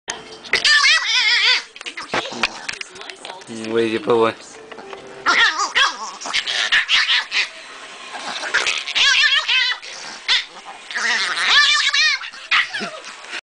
hurt/angry dog funny lol
this dog is anger but is wimperin like she is hurt